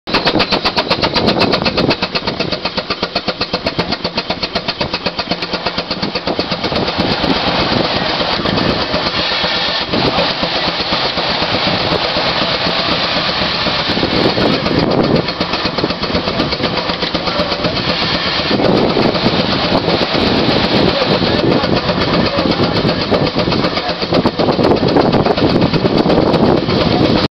24 hp Greyhound steam engine video4
1921 Greyhound 24 horse power operating on a final preparation prony brake.